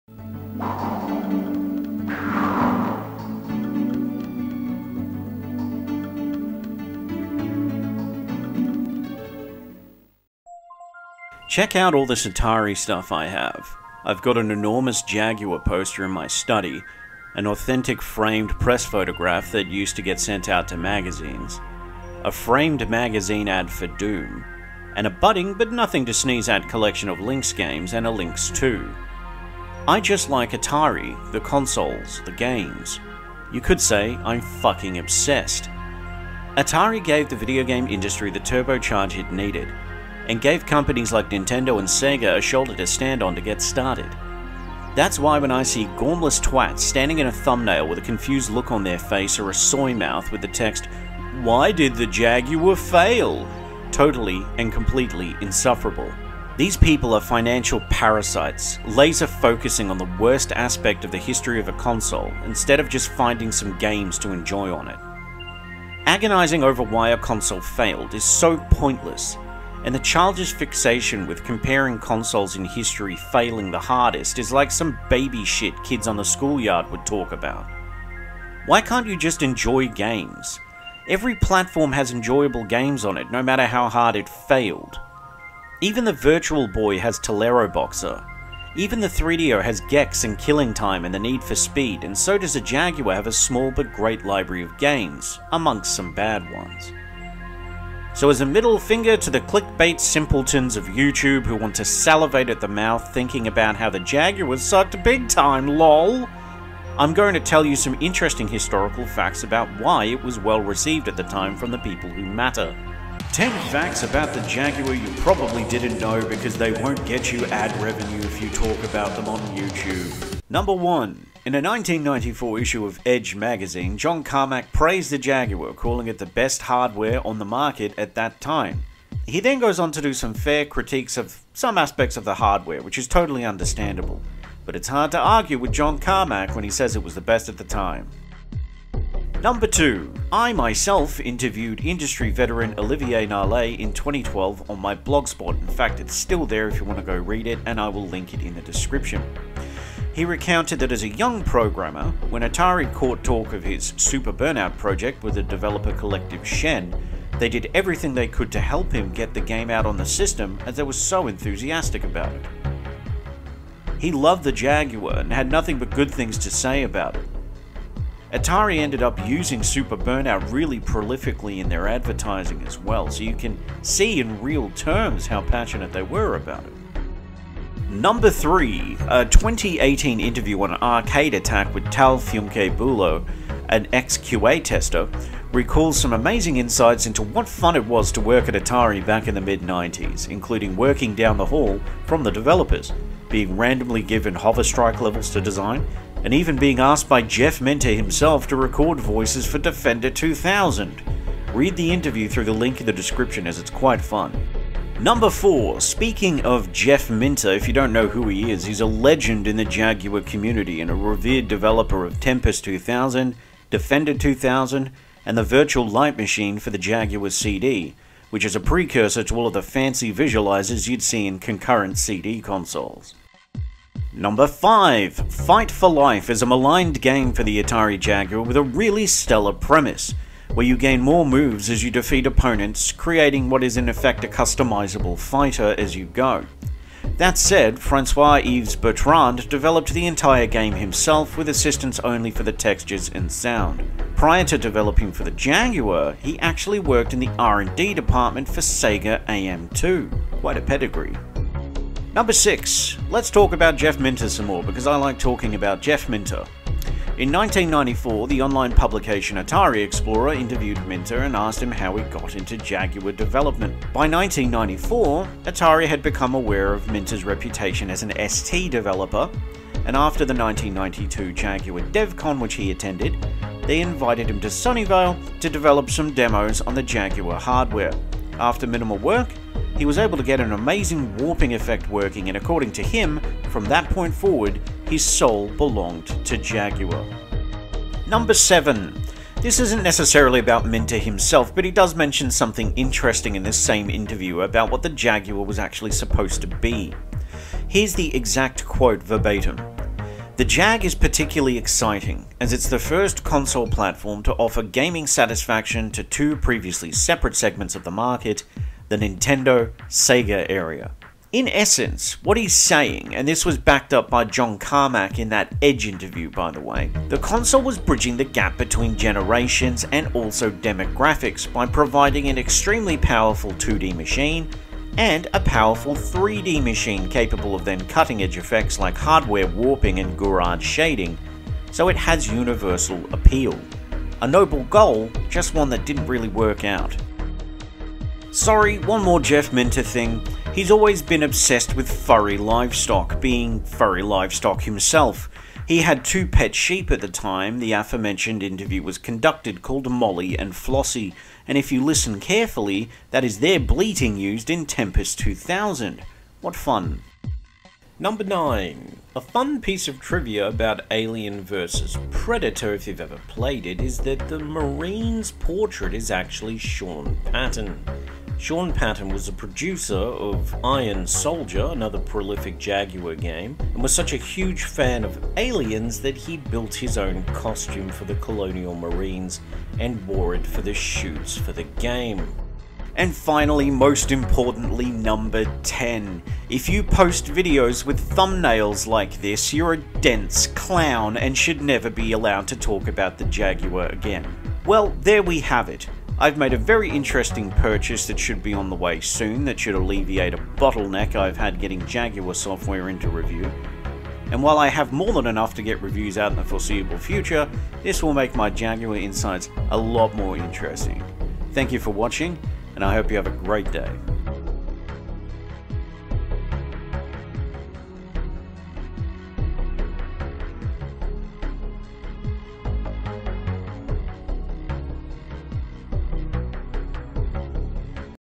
10 Facts About the Atari Jaguar You Probably Didn't Know Because They Won't Get You Ad Revenue
Hello everyone and welcome to a spite filled list of interesting facts about the Atari Jaguar because unlike other Youtubers, I actually care about games instead of gaining clout by parroting inaccurate, ignorant and hyperbolic fallacies about the console simply to generate clicks and ad revenue.

Only watch this if you're actually passionate about video games and don't watch this if you want to see a girl with her cleavage showing or a guy with his mouth open wide enough to swallow a car.